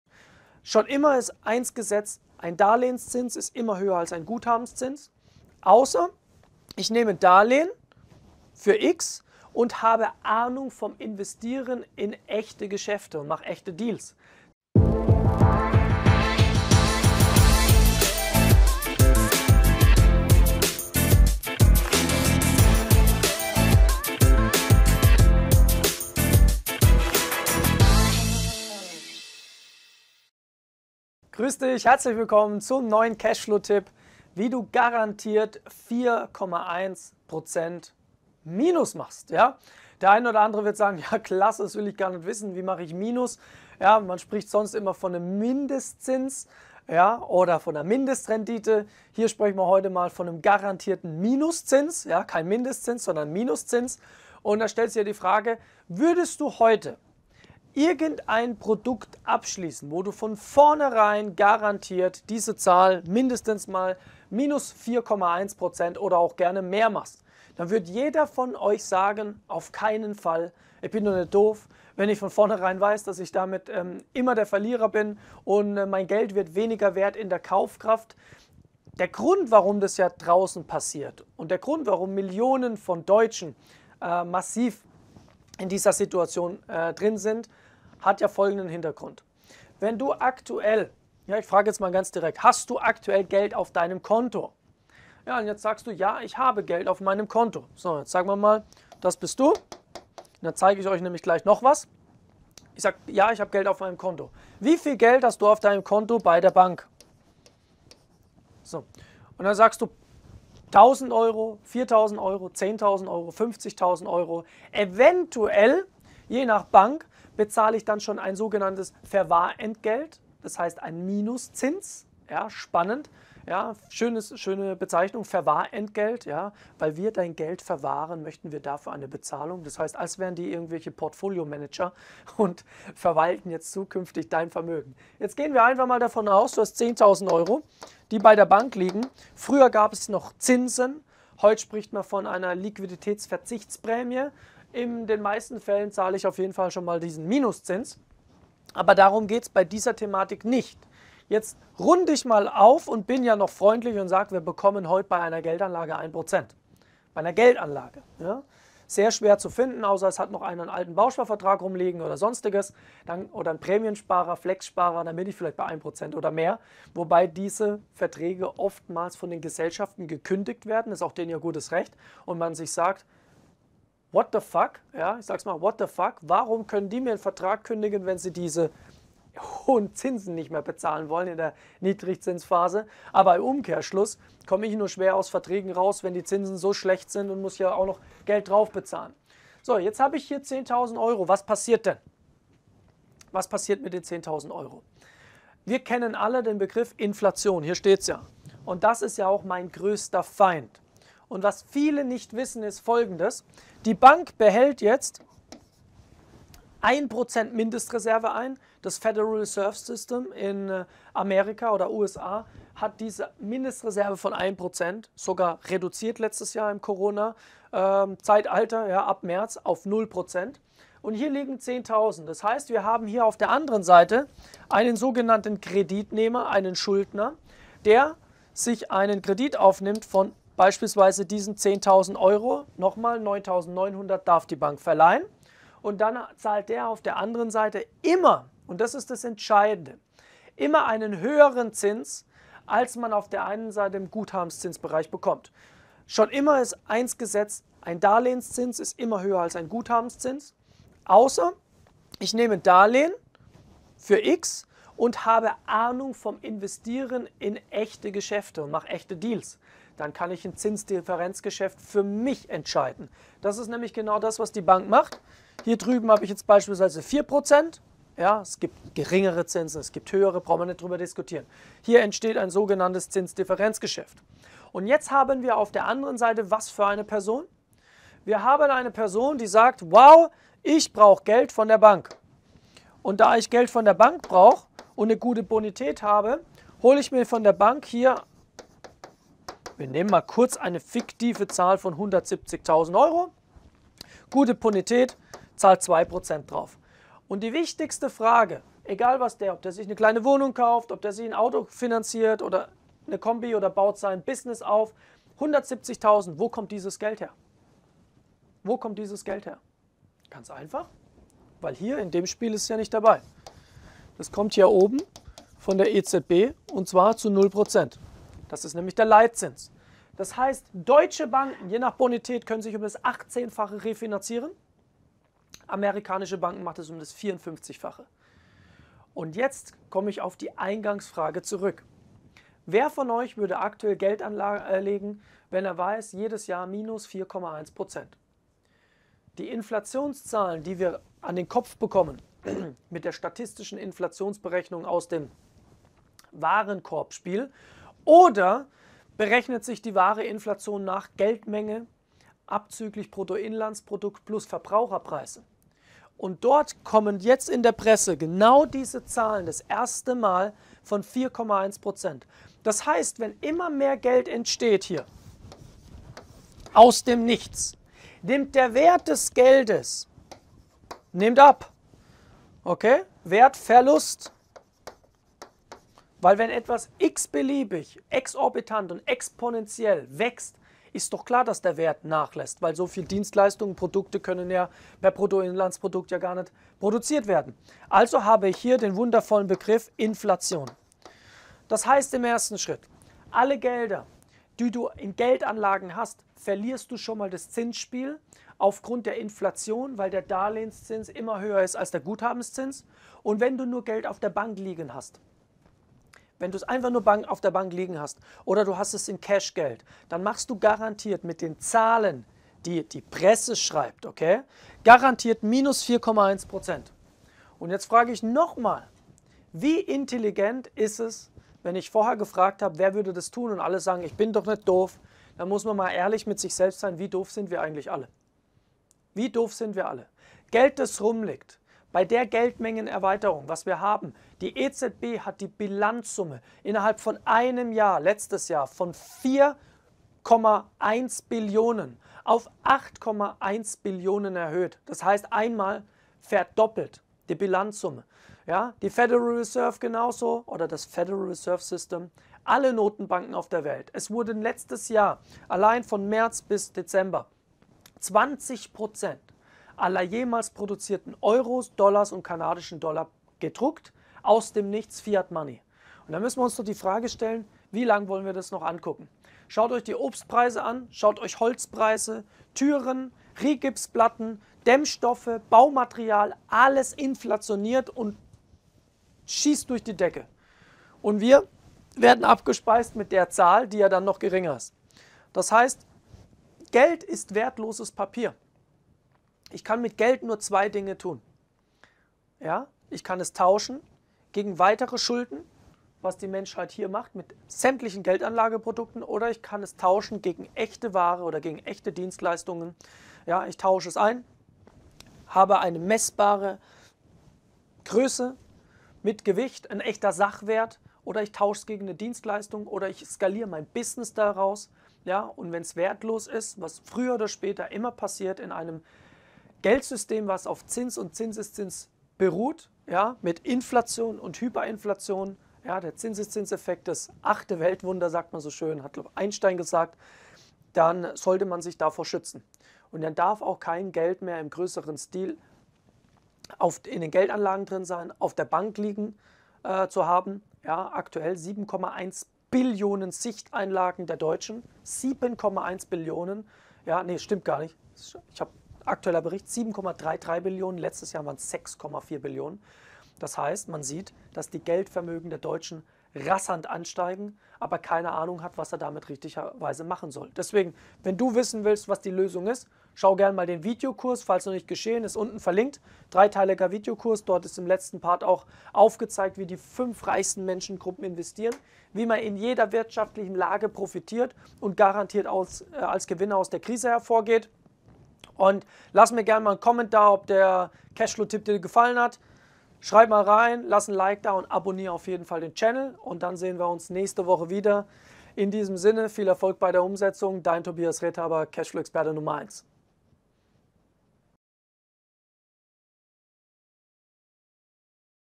Wie Du den Minuszinsen entkommst! (+Geld sicher anlegen)
Würdest Du Dein hart verdientes Geld in ein System mit einem garantierten Minuszins von - 4,1 % anlegen?

"Niemals", würdest Du sagen. Leider ist genau das die traurige Realität für die meisten Deutschen. Wenn Du Geld auf dem Konto oder in irgendwelchen klassischen Anlageprodukten hast, dann wird Dein Vermögen garantiert nächstes Jahr mindestens 4,1 % weniger Wert sein.

Es wird immer mehr Geld aus dem Nichts gedruckt, daher sinkt der Wert des Geldes Jahr für Jahr. Das Papiergeld wird früher oder später in seine Ursprungsform zurückkehren, nämlich zum wertlosen Papier.

Willst Du eine Lösung für diese tickende Zeitbombe haben? Dann schau Dir mein Video an und Du erfährst, wie Du als Gewinner aus der kommenden Krise gehst und welche Form der Geldanlage wirklich Sinn ergibt.